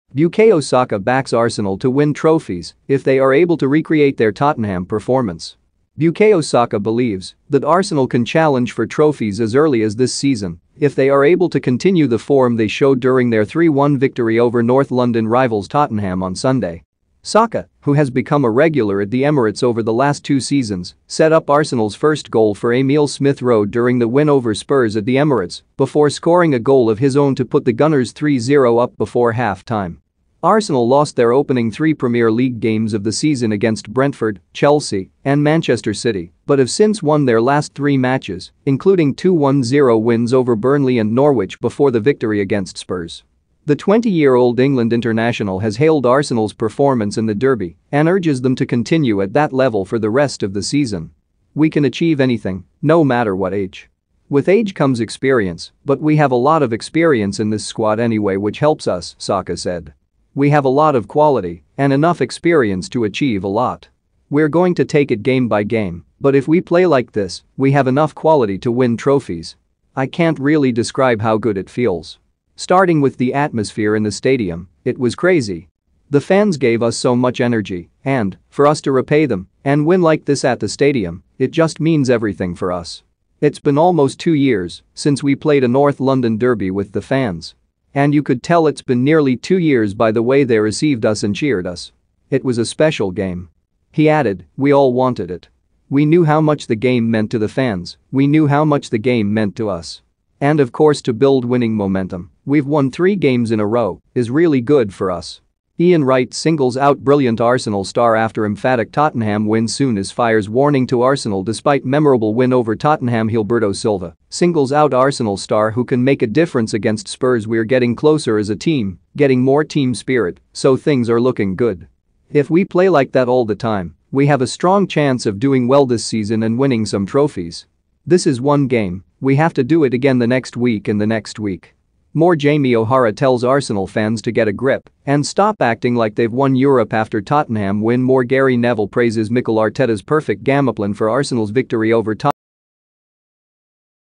Bukayo Saka backs Arsenal to win trophies if they are able to recreate their Tottenham performa
Bukayo Saka backs Arsenal to win trophies if they are able to recreate their Tottenham performance